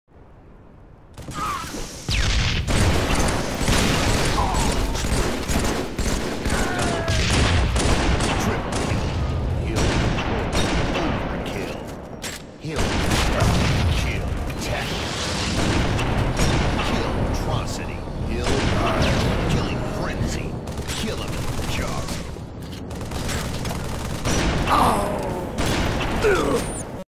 Reload glitch...
FAAAAAAAAAAAAAAAAAAAAAAAAK!
Got this a couple of weeks ago whilst playing with Dutteh. Coulda had the killtastrophe or mabye even pocalypse :( ah well
Expect some better quality uploads next week as I'm off for half term. Gonna make a leftovertage with actual good clips so keep your eyes out :D